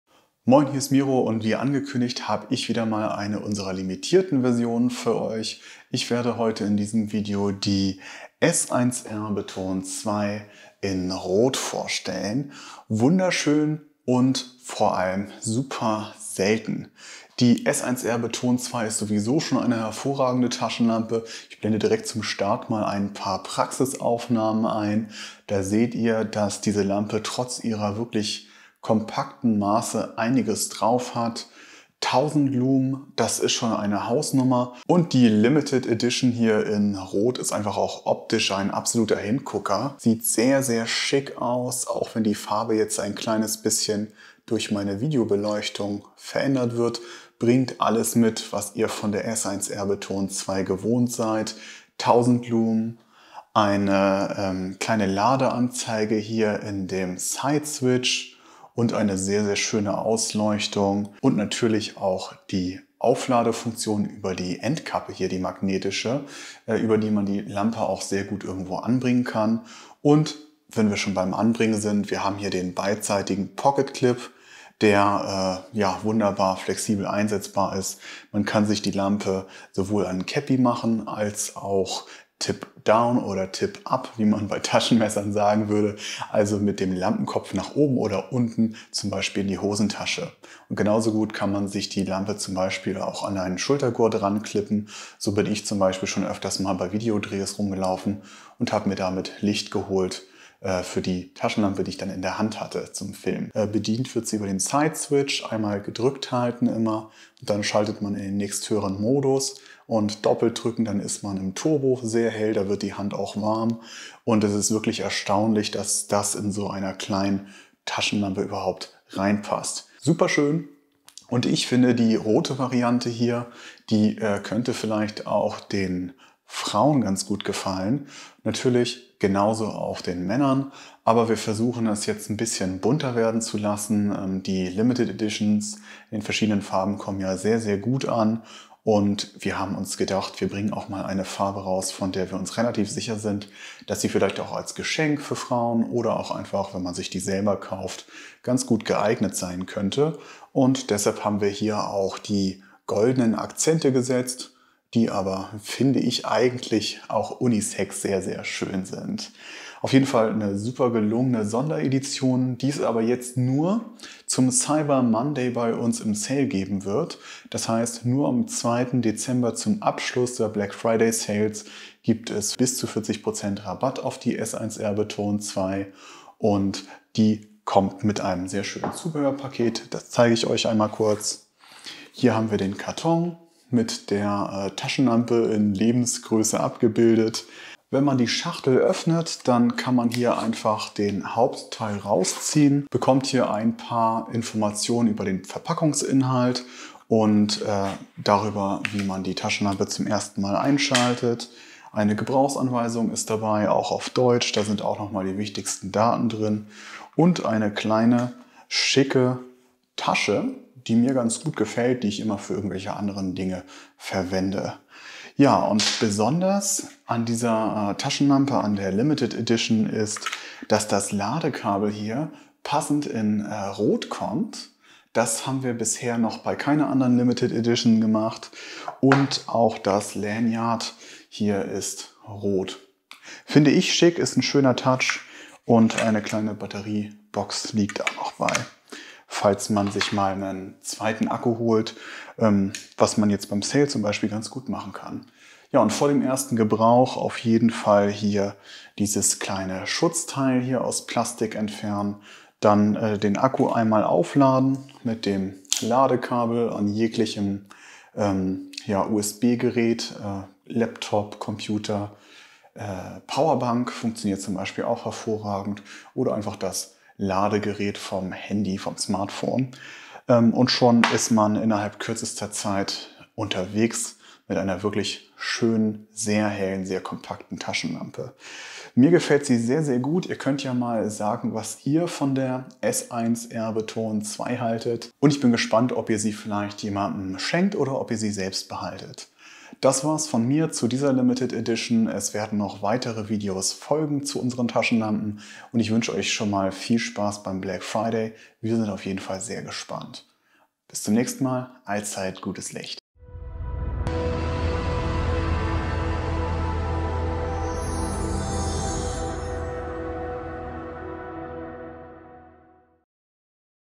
Olight S1R Baton II Red
Die S1R Baton II ist eine unserer beliebtesten Taschenlampen und das aus gutem Grund. Sie produziert 1000 Lumen und das im Hosentaschenformat!